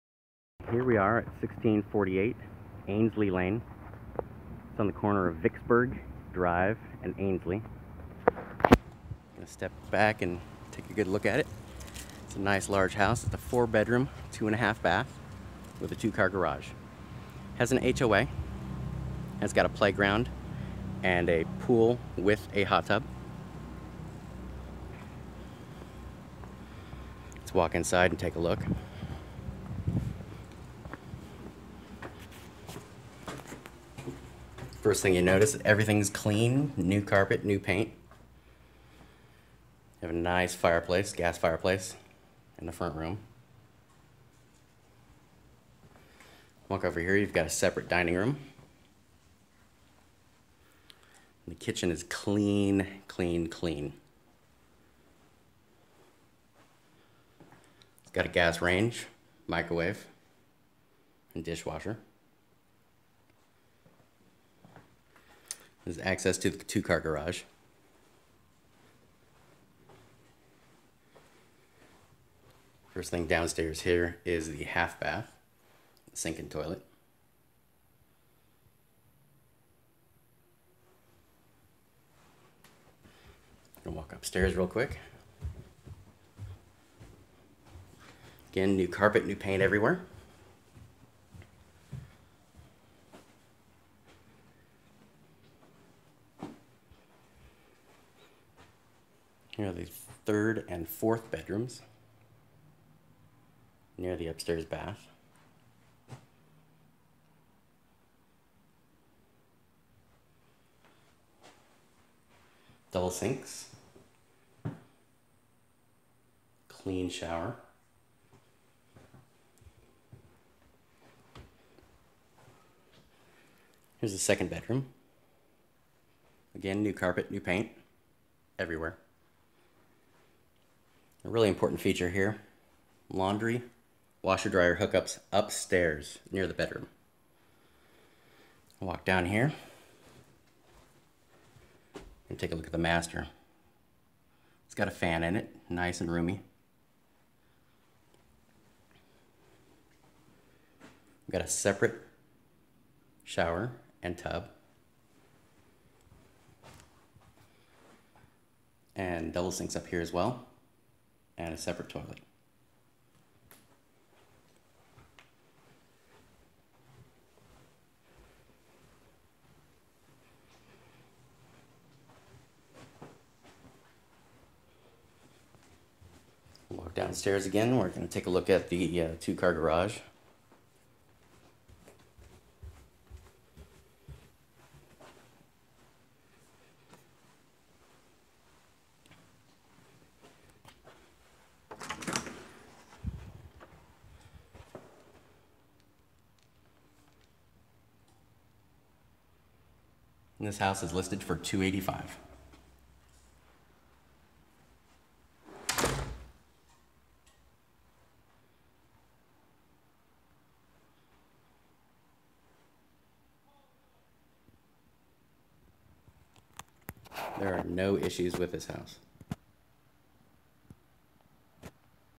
1648 Ainsley Ln, Fairfield CA Home for sale 285,000
Home for sale as of 8-28-15
4 bedroom 2 full bath and 1 partial bath. Very clean , updated with 2075 sq.ft of living space. Move in ready! It also has a community pool, hot tub and playground